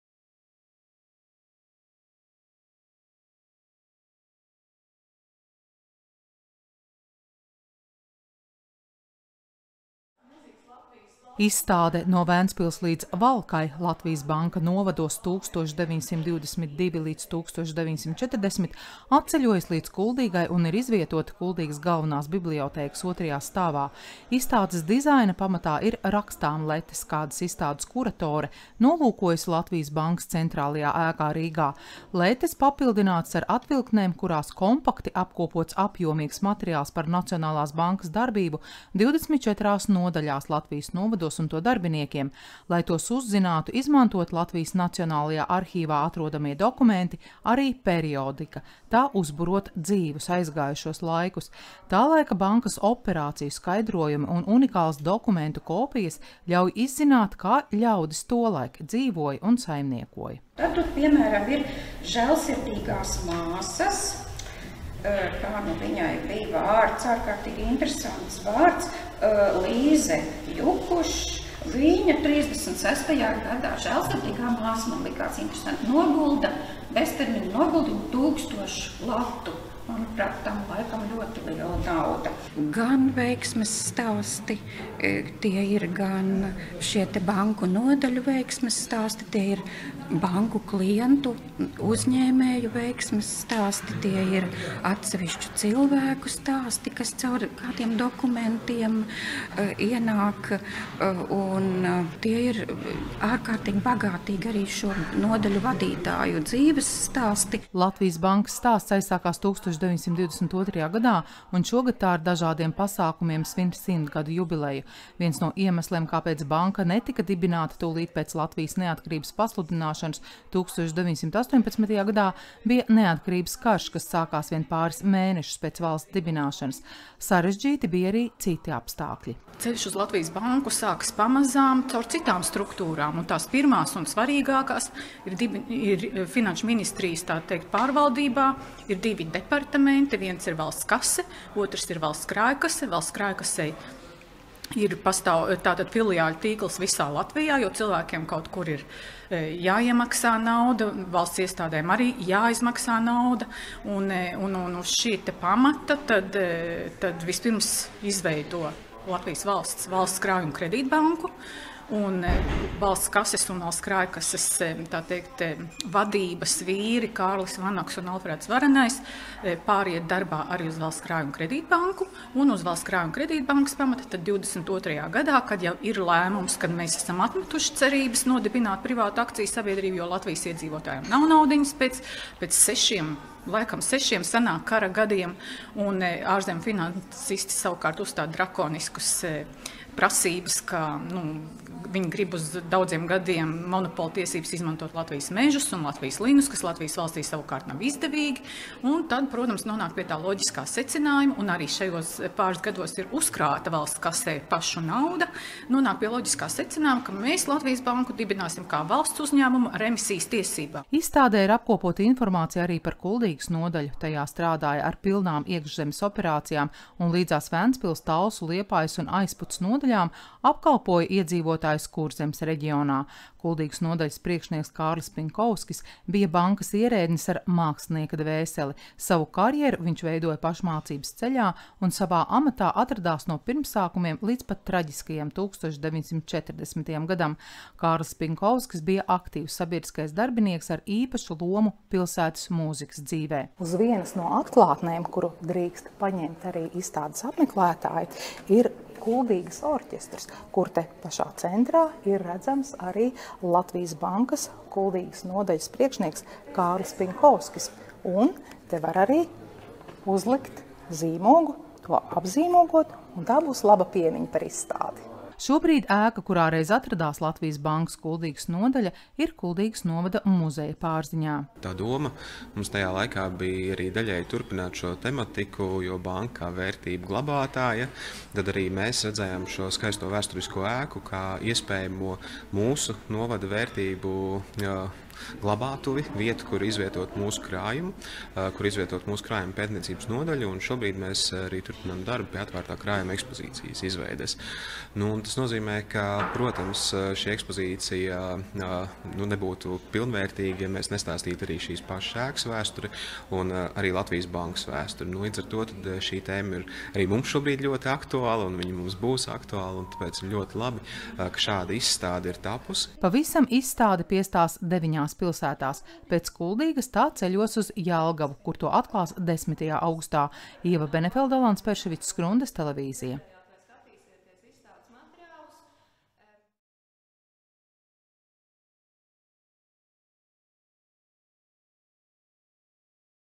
Latvijas Bankas izstāde Kuldīgā
Vēl līdz 7. augustam Kuldīgas Galvenajā bibliotēkā apskatāma ceļojošā izstāde “No Ventspils līdz Valkai. Latvijas Banka novados. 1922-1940”. Tajā iekļauts stāsts arī par Kuldīgas nodaļu un tās priekšnieku Kārli Pinkovski. Izstādē var iepazīties ar dažādos arhīvos un periodikā atrodamiem unikāliem dokumentiem un stāstiem, kas ļauj izzināt, kā ļaudis tolaik dzīvoja un saimniekoja. Turpina Skrundas TV.